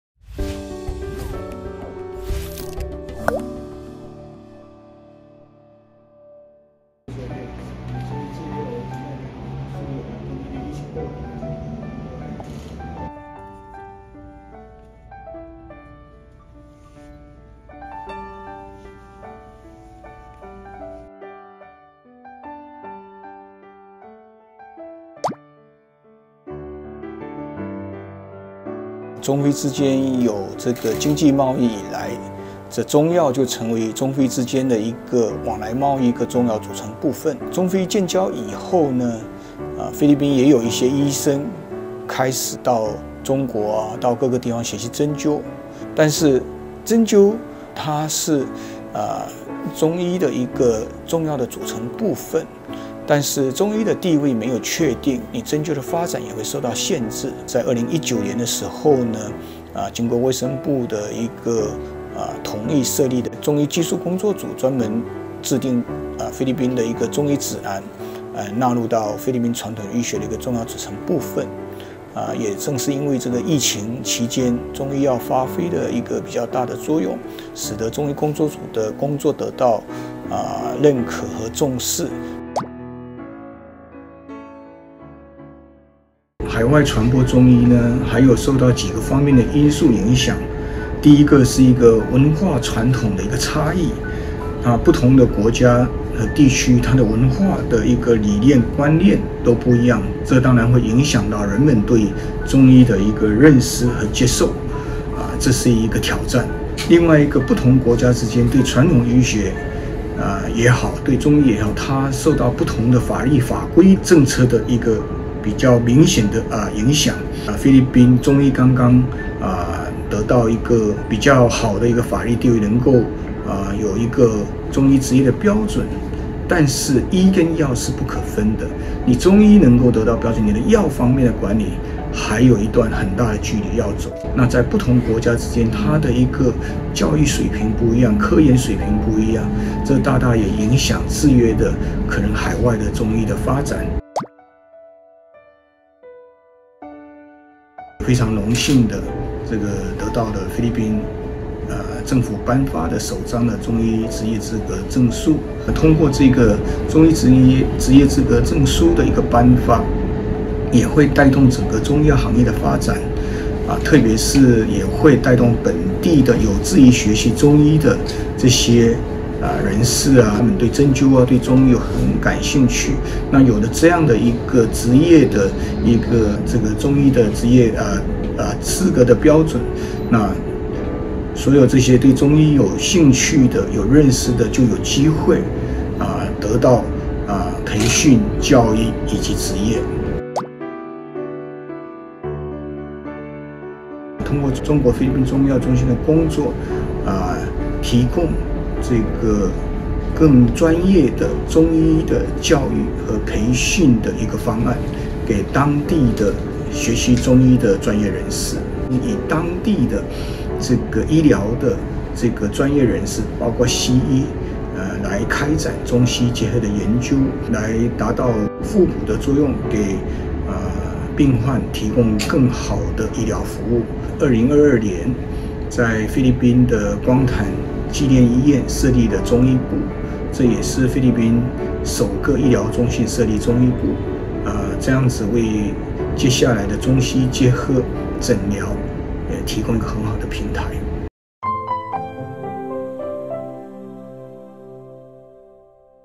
【东西问】郑启明：持证行医，中医何以在菲律宾获得官方认可？
2021年6月，菲律宾卫生部向中国—菲律宾中医药中心菲方主任郑启明颁发了该国首张中医执业资格注册证书，而该中心也正式成为菲律宾第一家持证行医的中医诊所。彼时，业内人士称这标志着中医药“下南洋”迎来新的突破。菲律宾首张中医执业资格注册证书的颁发有何特殊背景？对中医在该国的发展又意味着什么？菲律宾民众对中医的认可度受到哪些因素影响？近日，中国—菲律宾中医药中心菲方主任郑启明接受中新社“东西问”专访，就此作出解答。